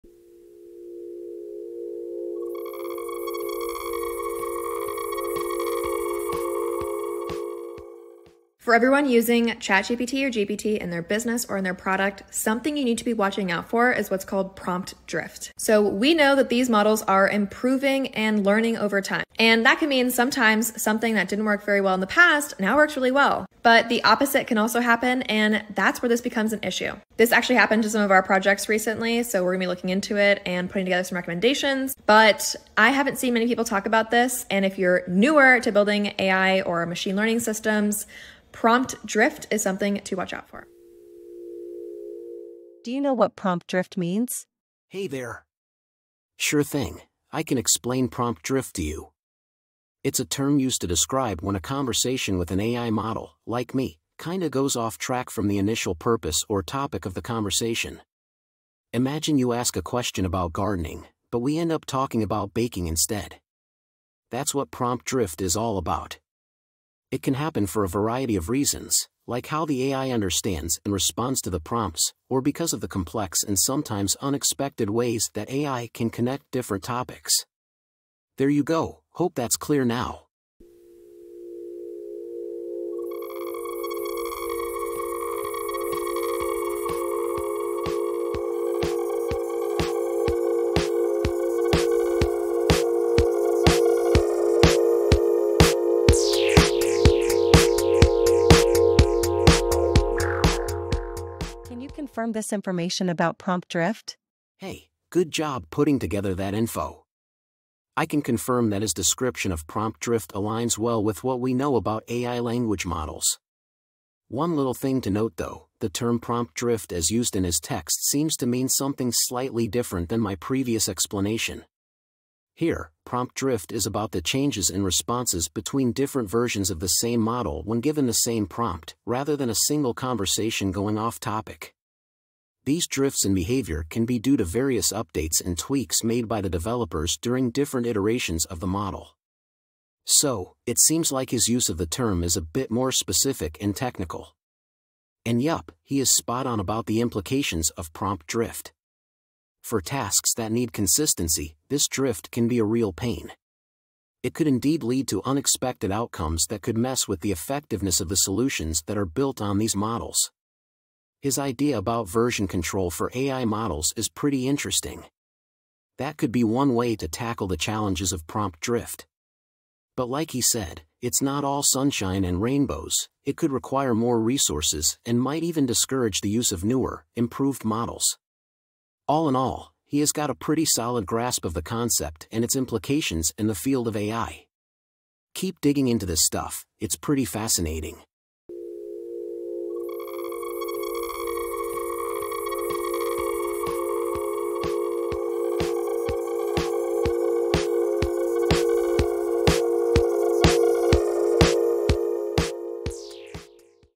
PROMPT DRIFT
Gm.
So, today I was deep in my work and I happened upon something called PROMPT DRIFT.

On one side, there was @therachelwoods  video, and I totally get how annoying it is when something like a PROMPT DRIFT happens. On the flip side, it was Rasool Alsakkaf, MSc ( Technology Management), who was picking up on what Rachel was talking about in her video.

I kinda knew this sort of thing could go down, but I didn't have a name for it. Like, when I was preparing the prompts for the INS[AI]DE video a year ago (Summer of 2022), it got this distinct dystopian, slightly artsy but also kinda odd early Midjourney Official style (must've been v3 or earlier). I made the installation-video, wrapped up the exhibition, and honestly, I was totally into its weirdness. But then, months later, in November 2022, when I was putting together my dossier on this project, I prompted some of the images again. This time it was V4. And wow, they changed a whole lot. You could really see the impact the updates to the Midjourney algorithm had on the outcome.

Rachel's right, you gotta be mindful of this. It can save you a lot of heartache!

So, I thought, why not make a video to test if other AI tools have also changed or improved since I last used them? Check out the video and enjoy. Now we all know what PROMPT DRIFT means!

Much love!

// Thanks to David Fisslthaler being the Avatar and @Tenchu Georg Tenchu for the music //

PS: check out DavidHs Wednesday office hours Discord. There you get the newest updates.
Links:
Rachel's Video: